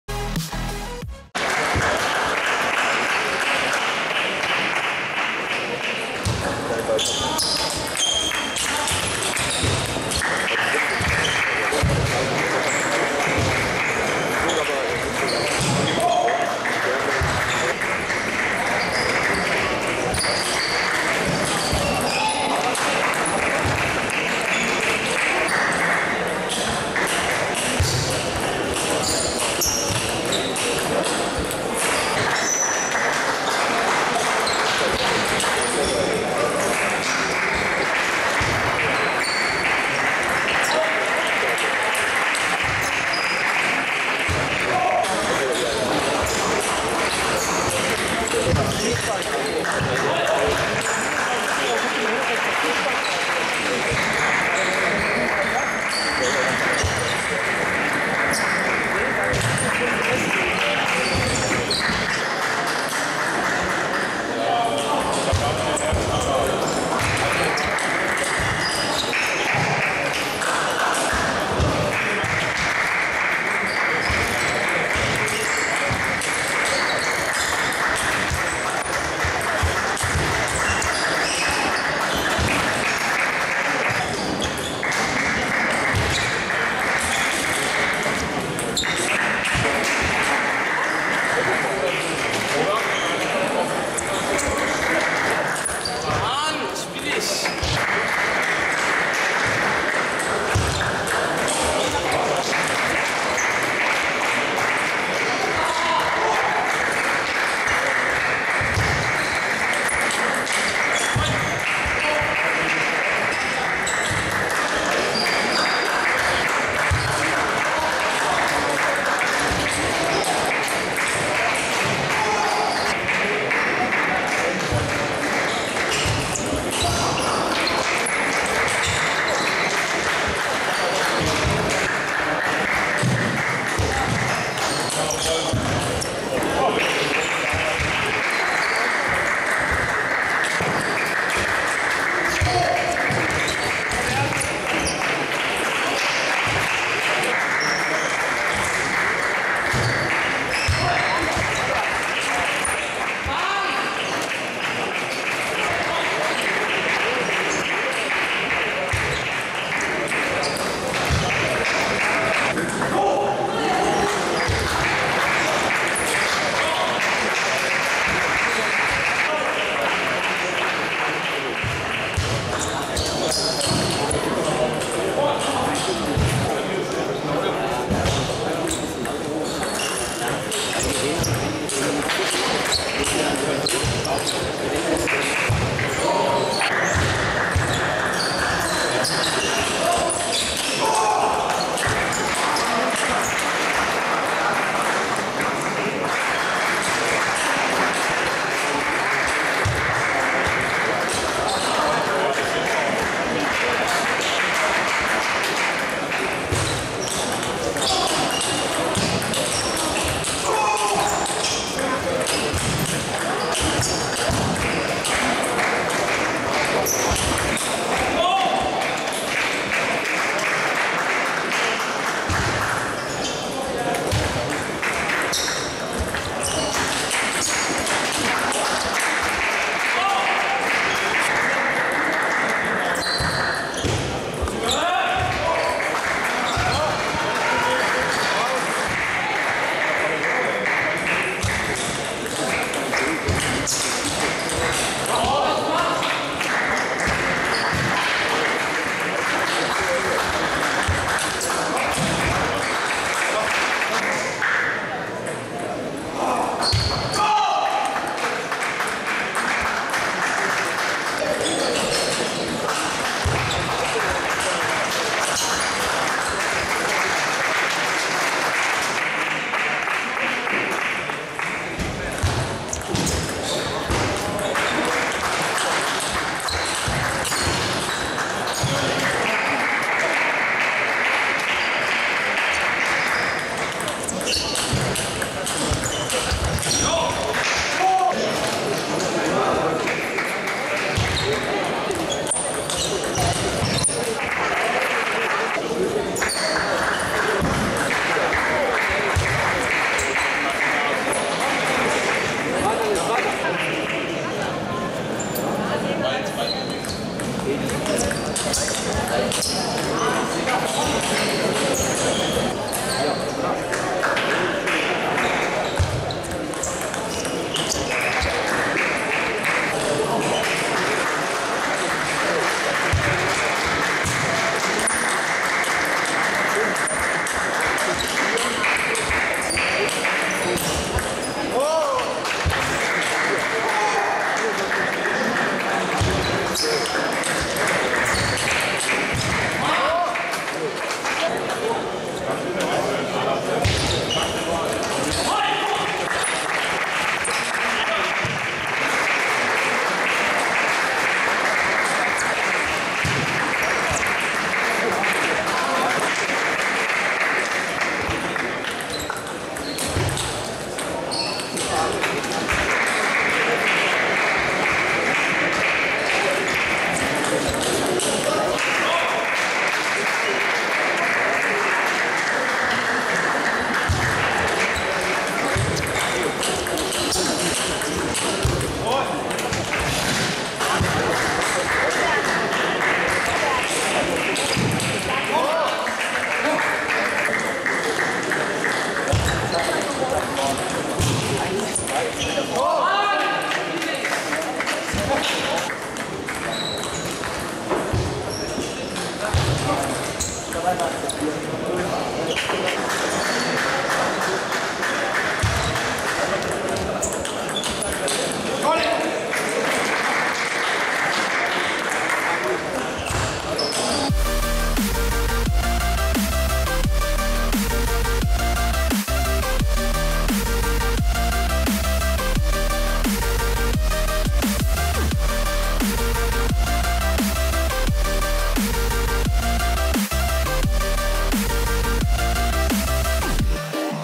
Timotius Köchling - Timothy Franke | DEUTSCHE MEISTERSCHAFTEN 2018
21.04.18 | Deutsche Jugendmeisterschaften | Berlin | männliche Jugend | Gruppenphase | Timotius Köchling - Timothy Franke  | Highlights

Großer Dank geht raus an den SC Siemensstadt für die tolle Durchführung des Turniers!